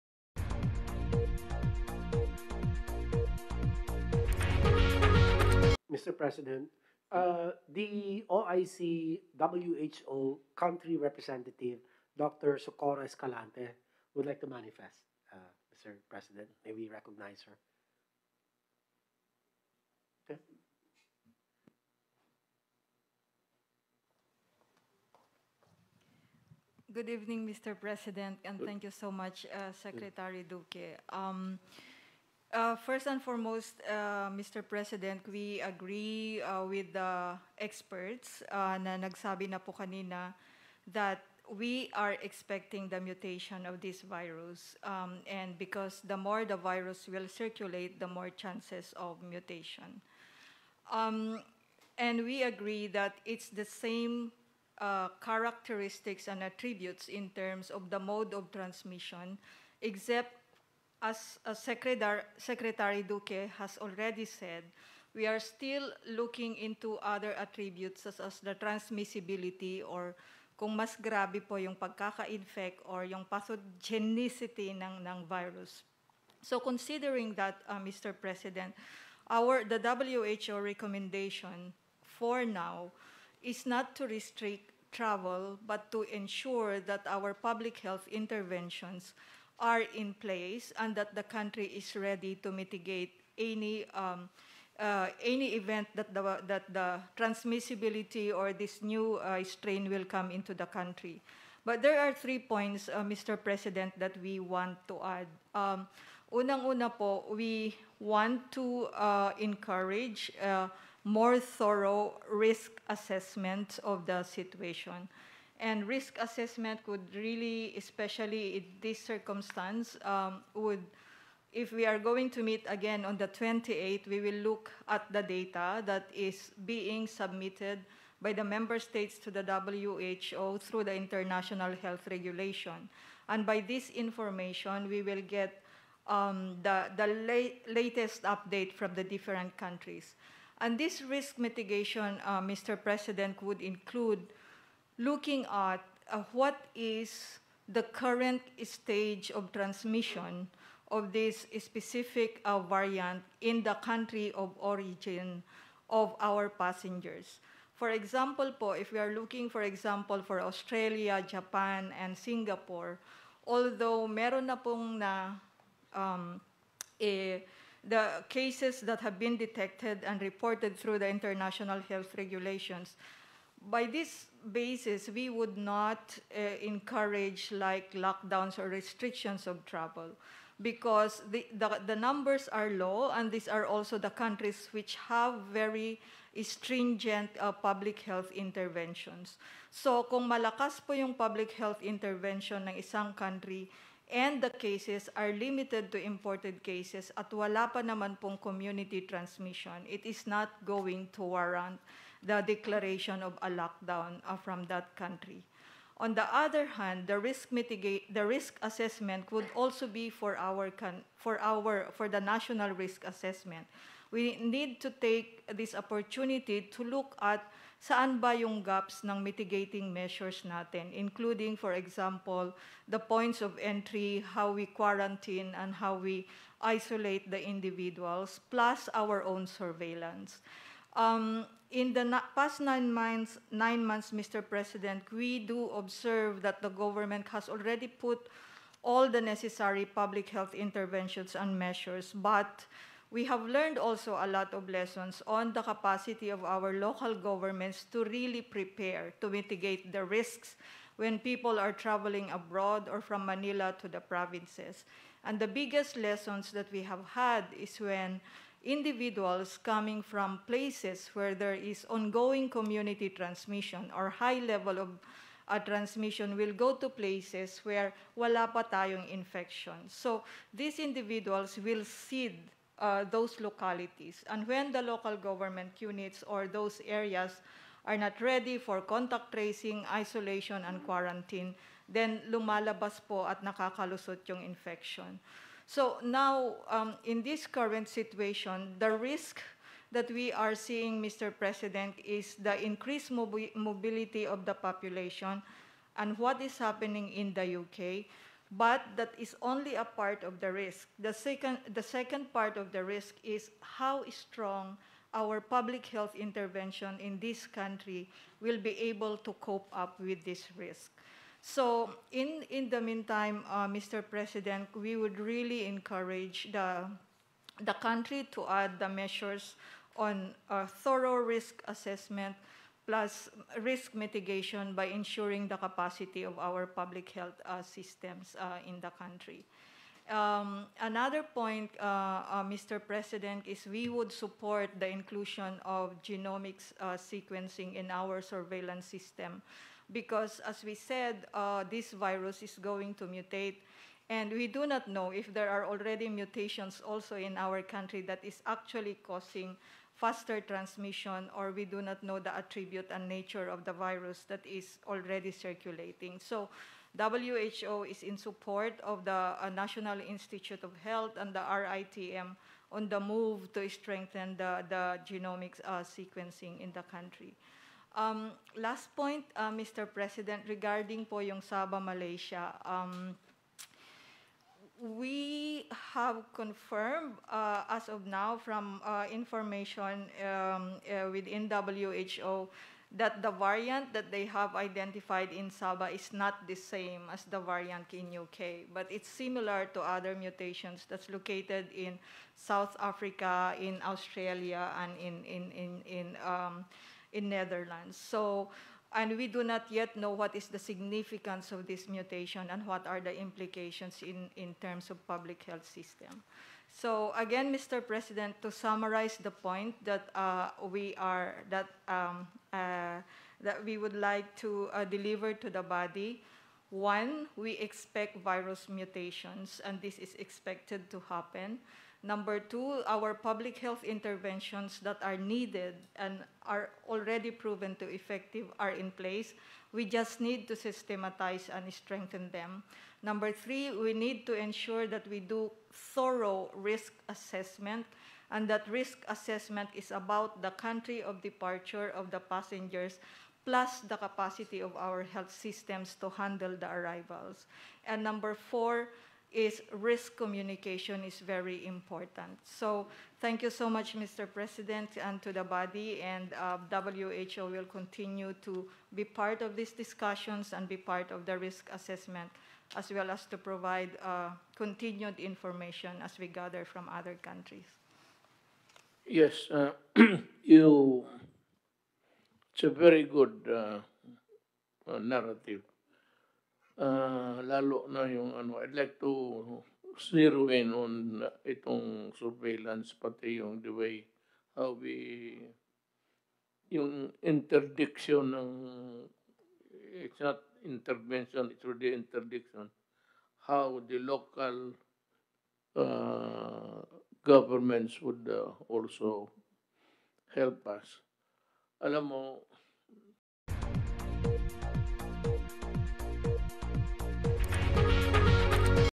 DR SOCORRO ESCALANTE: CONTROL OF THE TRANSMISSION IS IMPORTANT IN STOPPING VIRUS MUTATIONS
World Health Organization’s Coordinator for Essential Medicines and Health technologies Dr. Socorro Escalante said control and suppression of the transmission are important in stopping virus mutations.

"It will be more difficult may be if these mutations are very important in terms of increasing transmission or infectivity or even the severity of the illness then it will be more difficult for us to fight Covid-19," Socorro added.